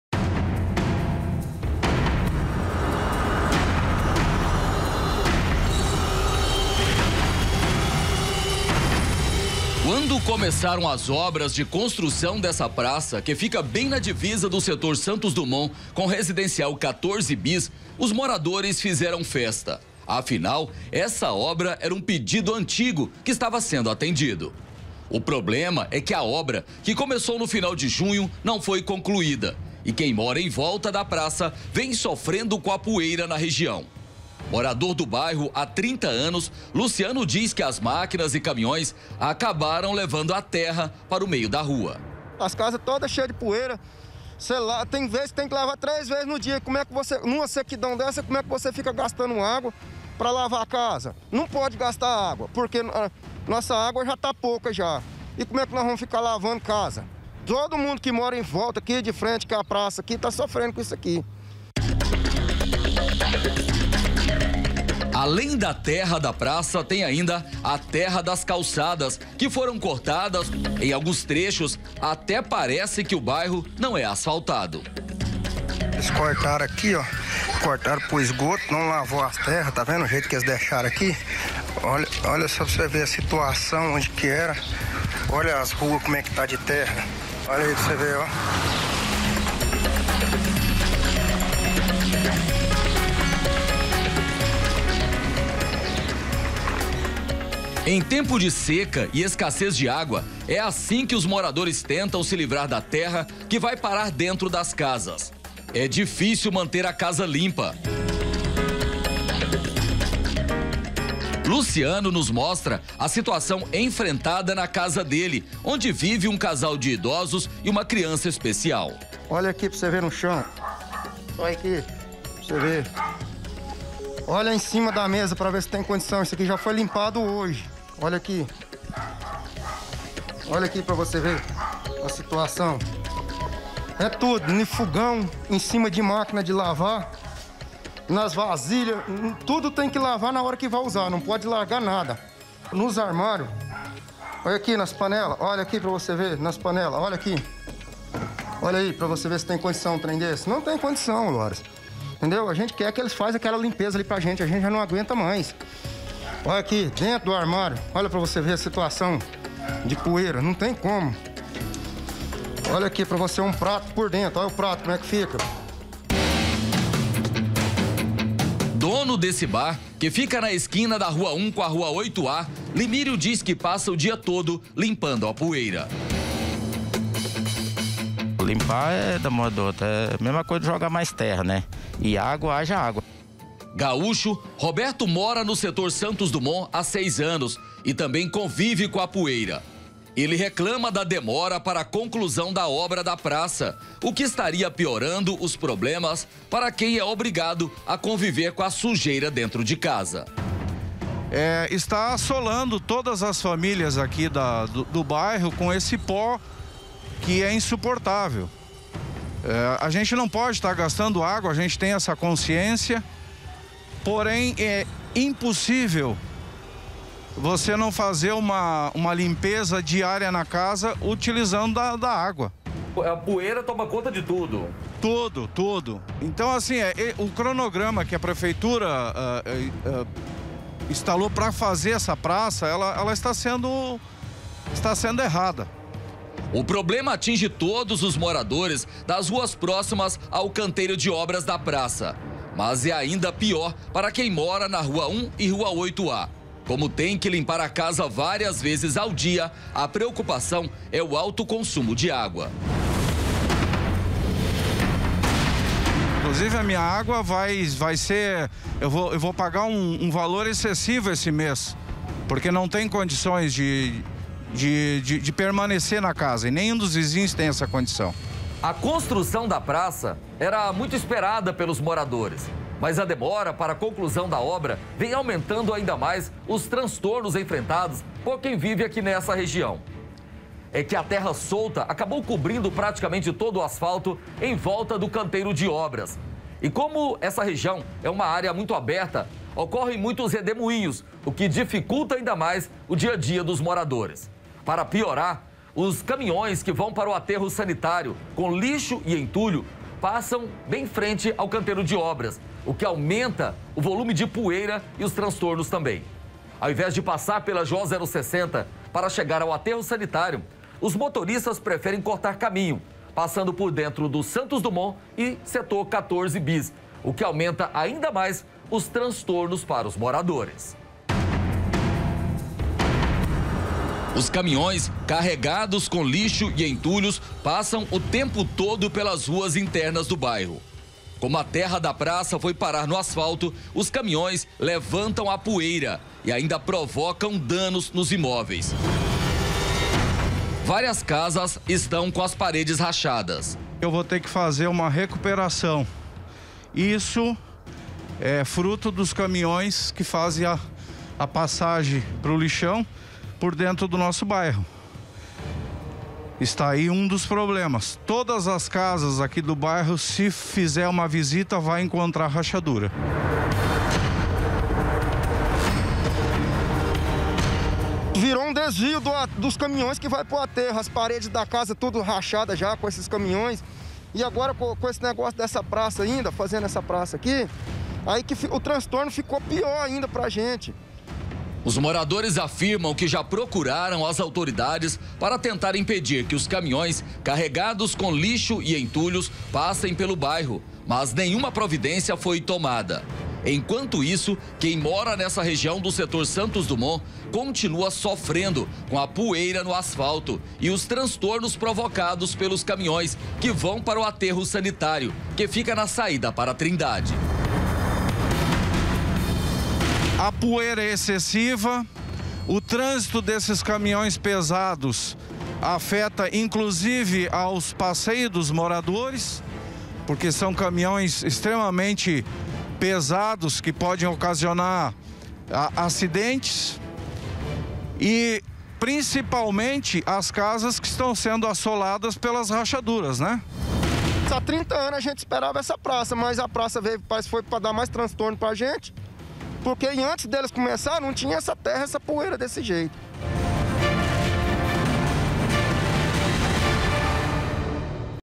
MORADORES RECLAMAM DE POEIRA E COBRAM CONCLUSÃO DE OBRA DE PRAÇA
Moradores reclamam de muita poeira e cobram conclusão de obra da Praça Santos Dumont.